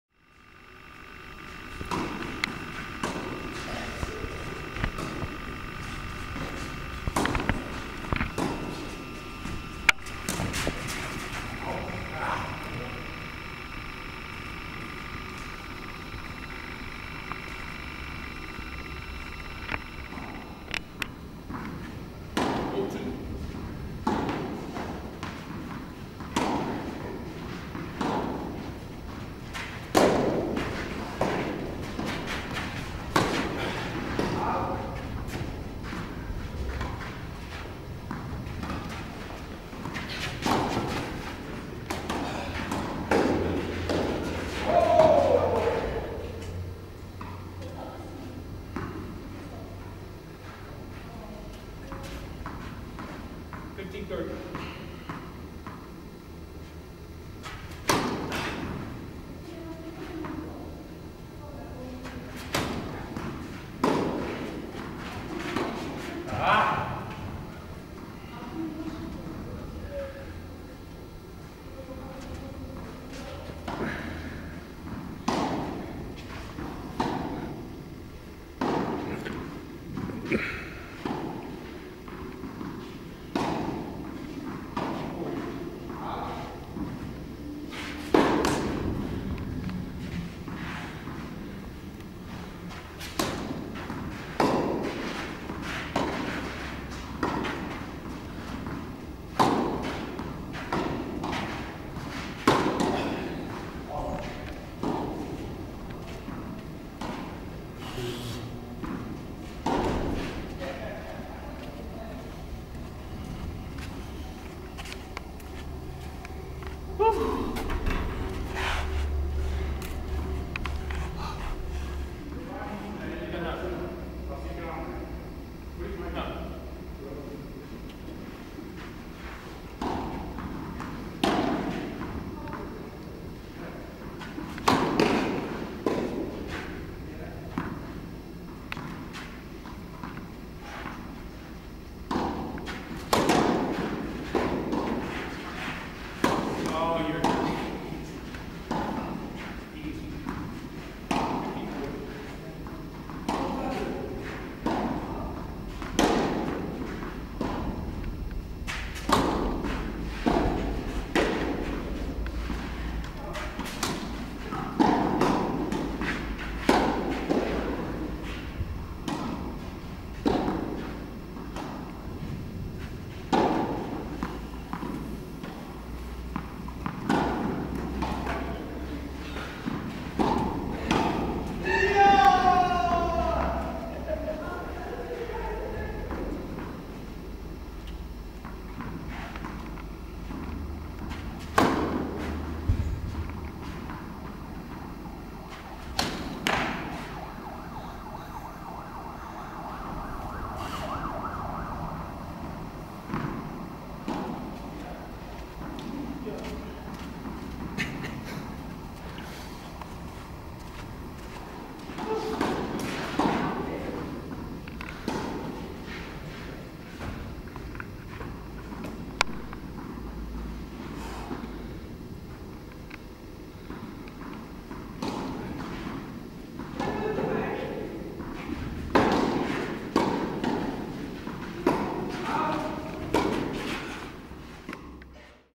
Marc tenis in Czech on red clay
Playing tennis in Czech Republic against , master of clay courts.